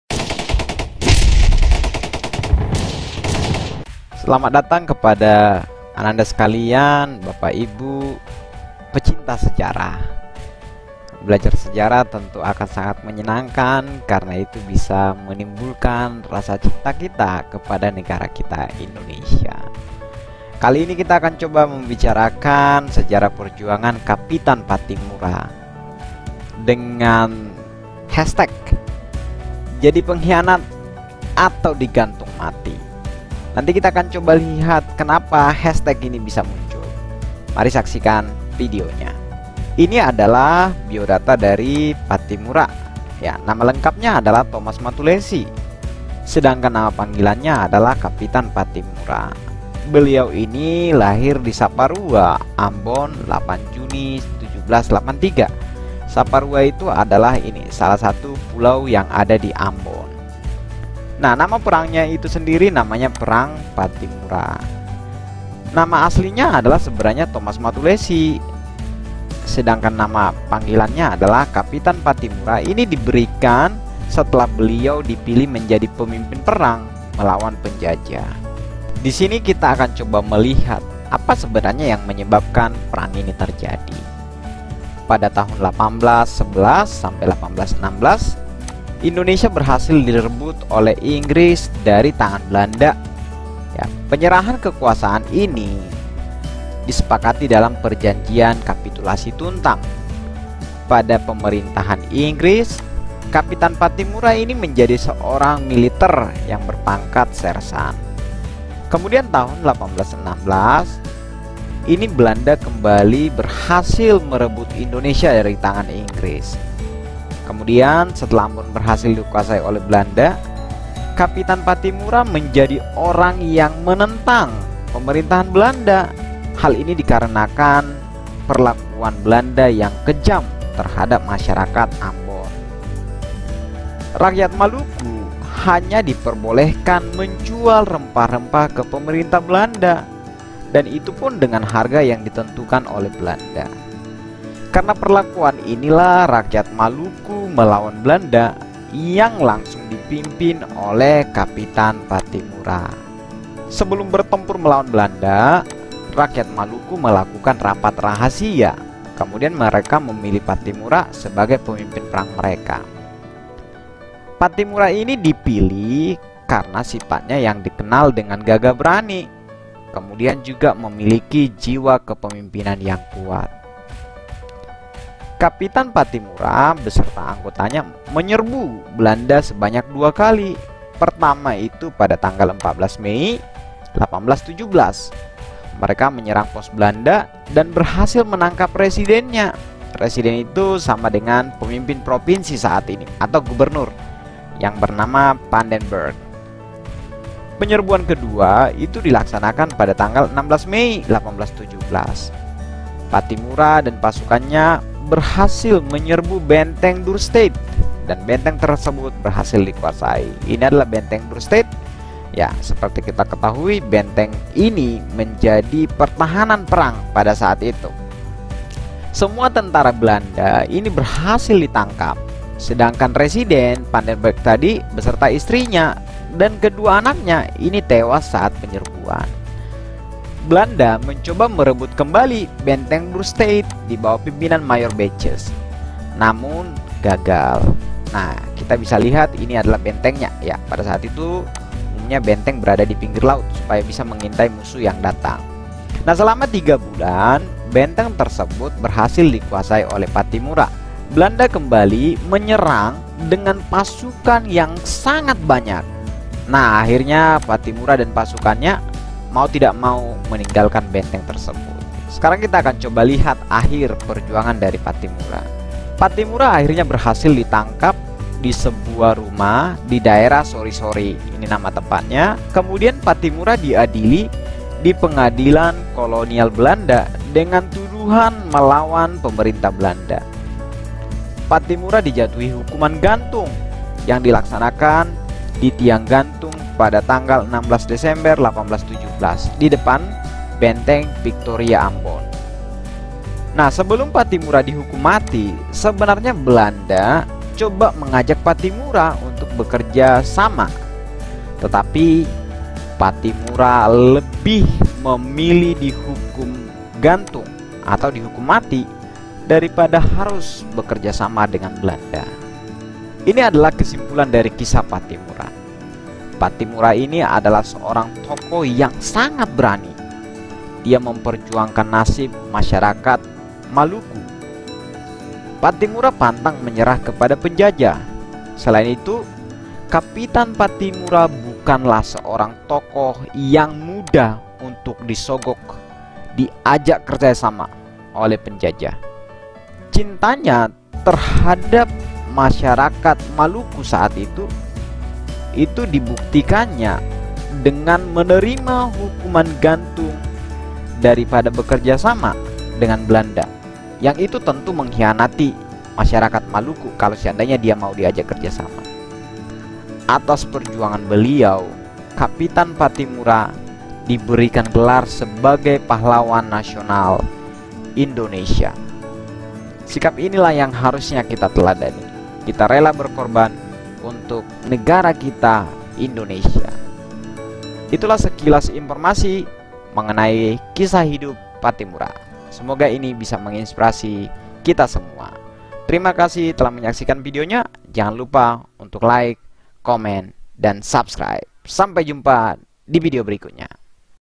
SEJARAH PERJUANGAN KAPITAN PATTIMURA
Pattimura salah seorang pahlawan Indonesia yang berasal dari Maluku. Bagaimana perjuangan Pattimura melawan penjejahan Belanda? Bagaimana kisah perjuangannya? Kemudian bagaimana akhir kisah hidup pattimura?